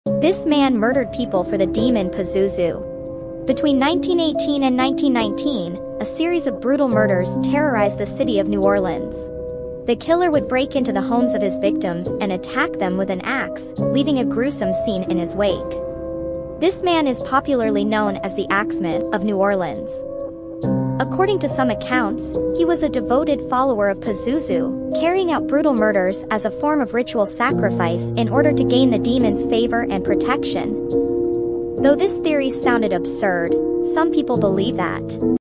The Axeman of New Orleans
The Axeman of New Orleans was a notorious and unidentified serial killer who terrorized the city of New Orleans in the early 20th century. His reign of terror began in 1918 and continued for over a year, during which he targeted mostly Italian-American residents, using an ax to brutally murder his victims in their own homes.